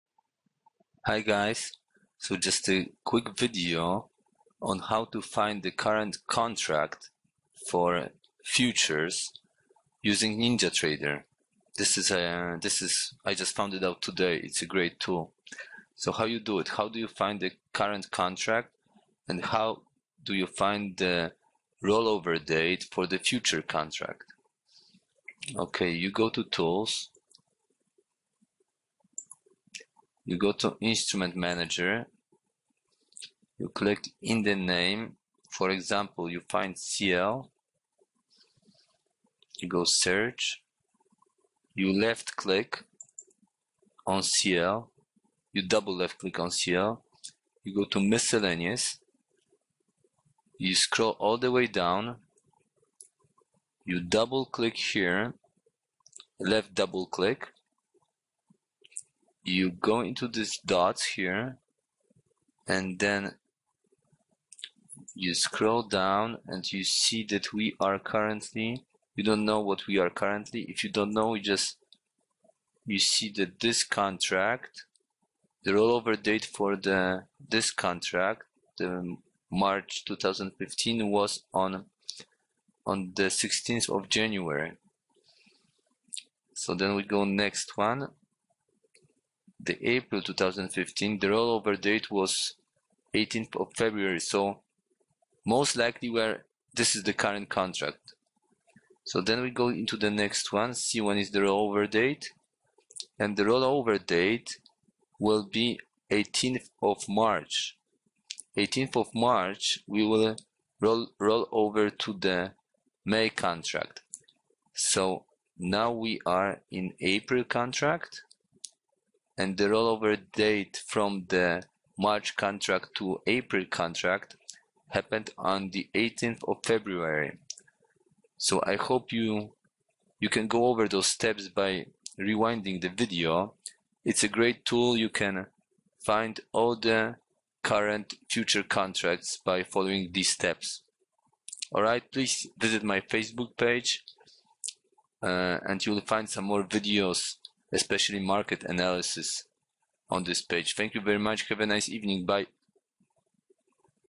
How to find the current future contract and rollover date on Ninja Trader!!!
Video showing easy way to find any current future contract and rollover date for the next contract using just Ninja Trader